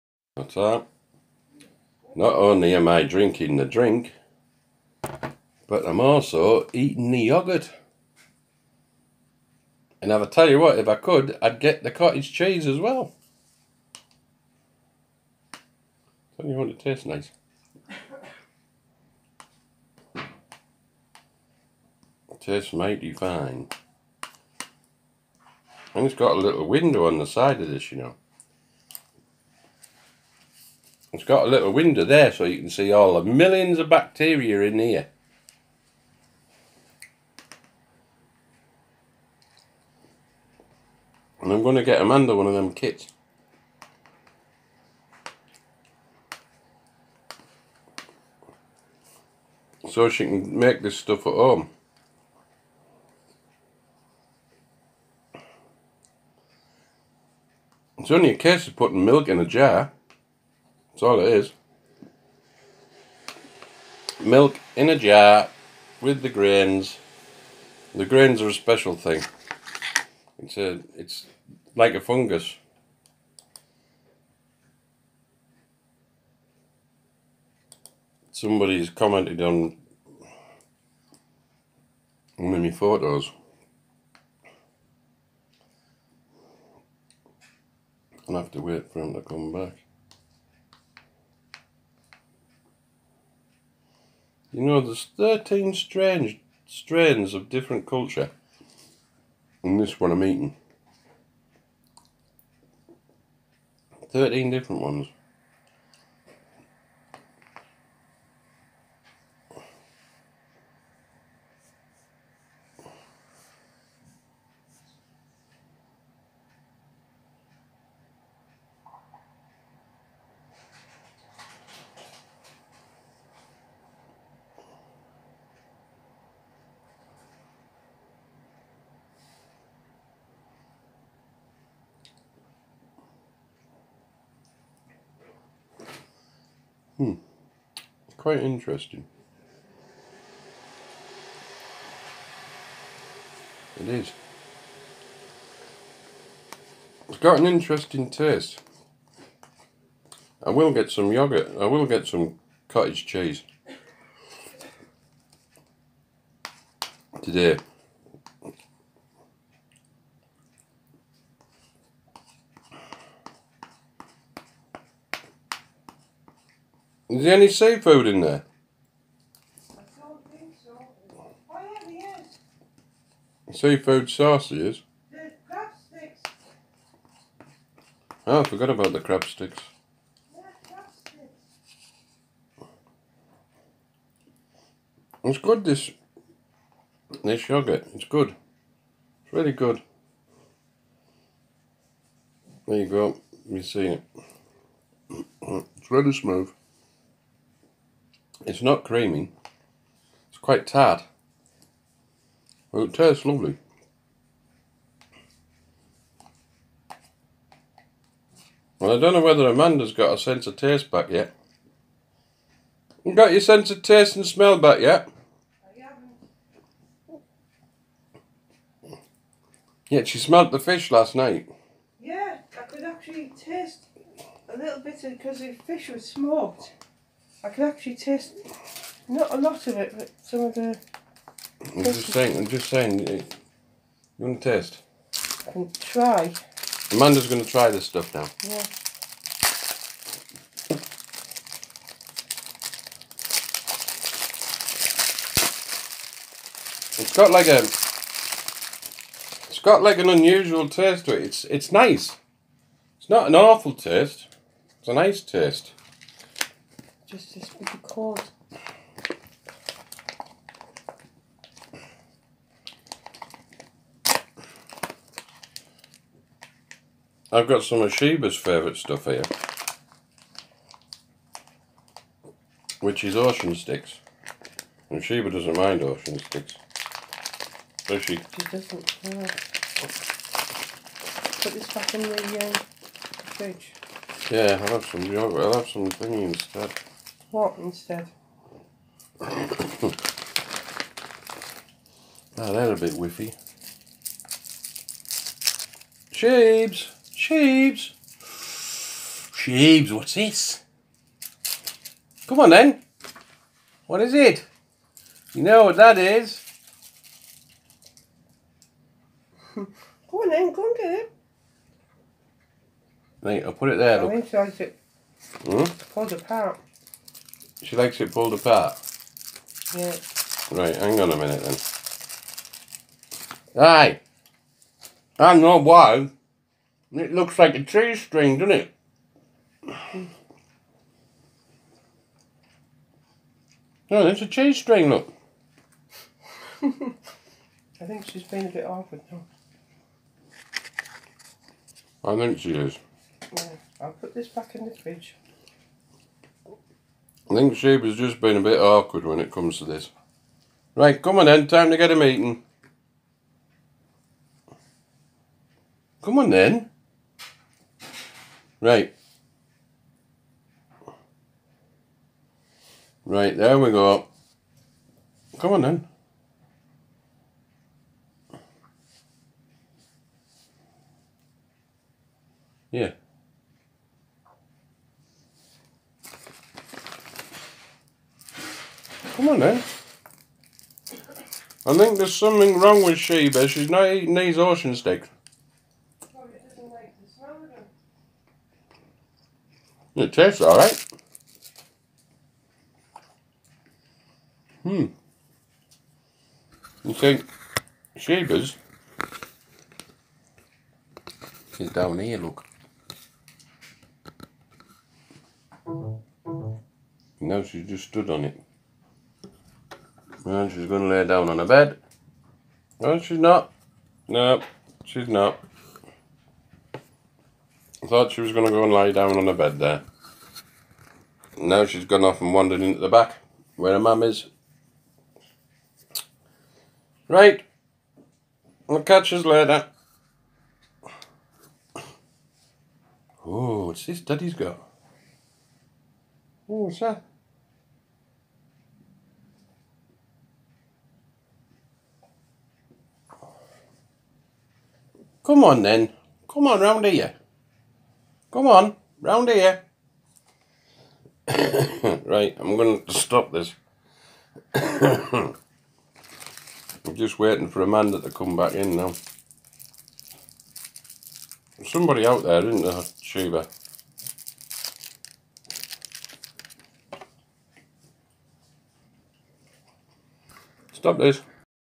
Ocean sticks kaffir yoghurt and drink
A description about bacteria in yoghurt and there's live milk drink they reckon you can get raw milk which I was brought up on as a kid because it was cold unpasteurized milk yeah unpasteurized milk well I've got the work out to come today I'm feeling so tired all I want to do is sleep which is not as good as one